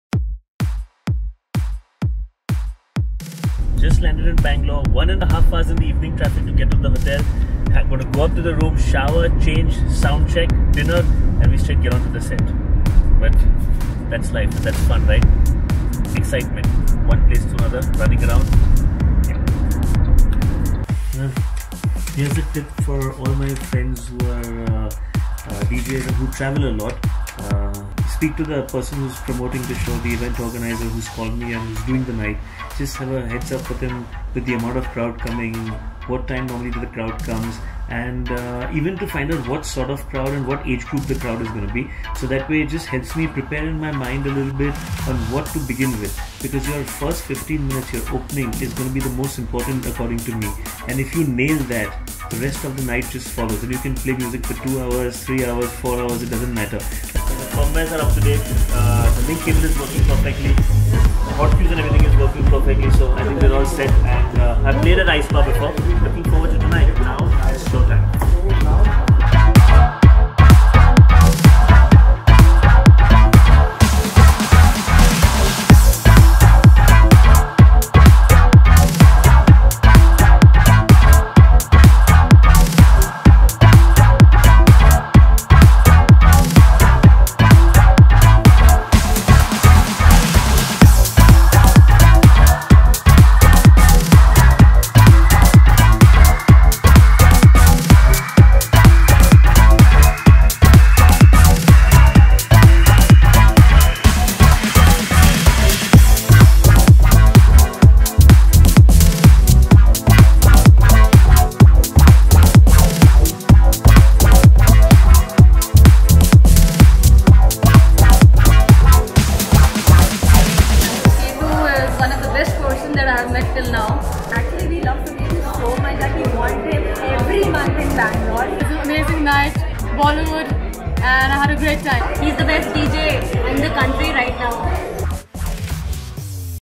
Partying in Bangalore - DJ Suketu Vlogs
DJ Suketu comes back to Bangalore for a solid night at Taj Ice Bar, MG Road.